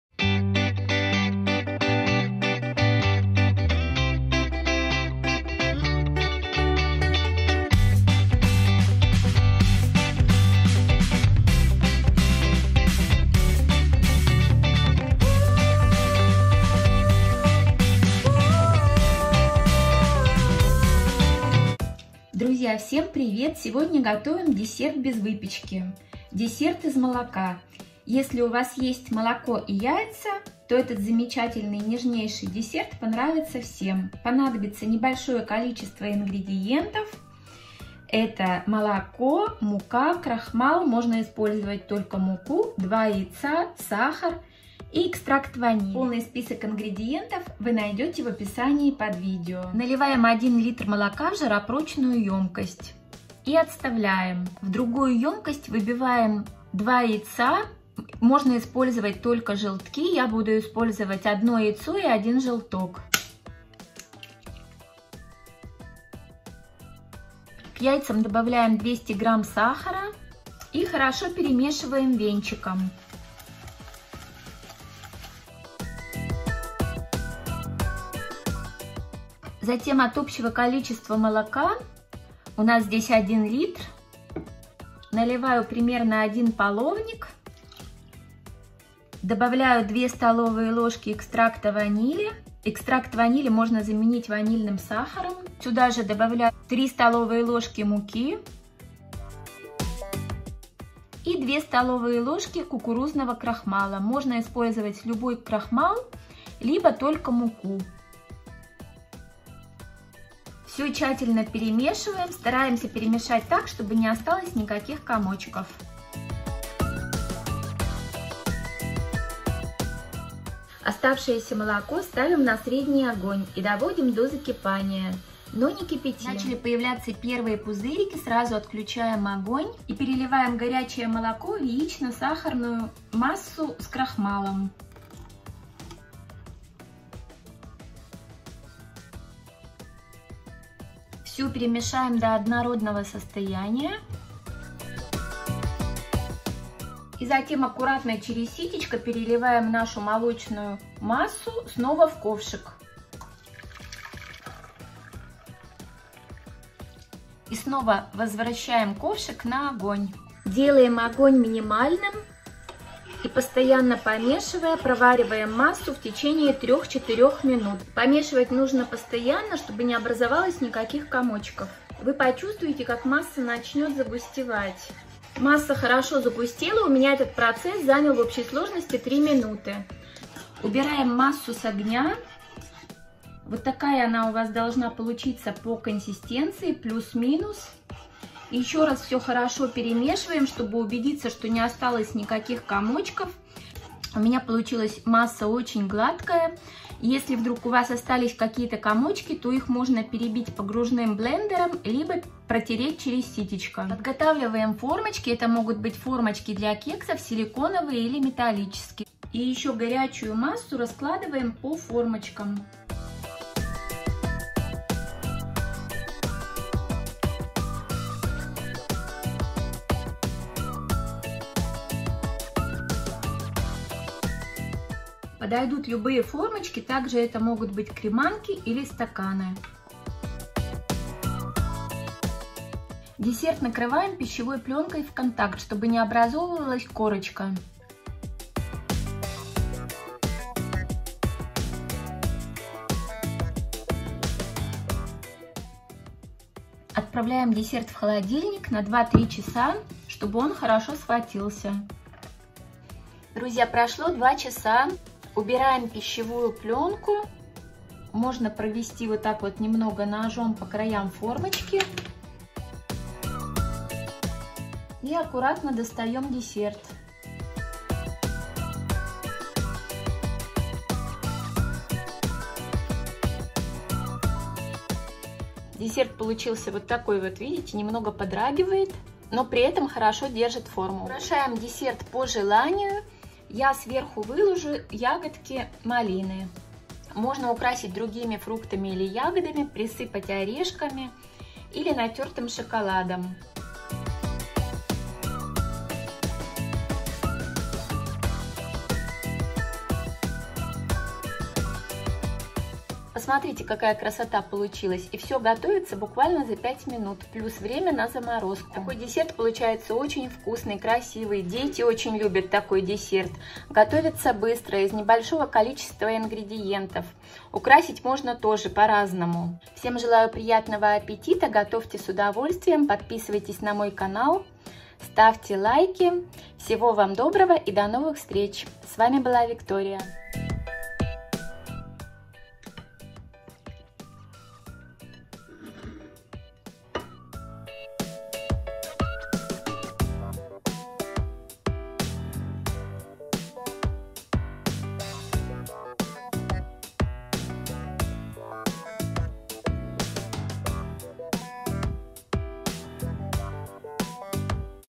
У Вас есть МОЛОКО и ЯЙЦА?Тогда приготовьте ЭТО !Десерт без выпечки Десерт из молока!
Друзья, сегодня готовим Десерт без выпечки. У Вас есть Молоко и Яйца Тогда приготовьте Это ! Десерт будет Без желатина Без духовки и Без выпечки ! Десерт из молока получается Нежный и Вкусный !

ДЕСЕРТ БЕЗ ВЫПЕЧКИ 🍰😍🥮🥛🍰
ДЕСЕРТ ИЗ МОЛОКА / ПРОСТОЙ РЕЦЕПТ 🍮

Друзья, У Вас есть МОЛОКО и ЯЙЦА? Тогда приготовьте ЭТО ! Нежнейший десерт без выпечки получается очень ВКУСНЫЙ и готовиться очень быстро !

Ингредиенты :
Молоко - 1 л, Яйца - 2 шт , Сахар - 180-200 г ( можно по вкусу ), Мука- 3 ст л , Крахмал - 2 ст л , Экстракт Ванили - 2 ст л ( можно заменить ванильным сахаром или палочкой ванили ).
На украшение : Любые фрукты или ягоды, тёртый шоколад или орешки.

Всем Желаю Приятного аппетита ! 👍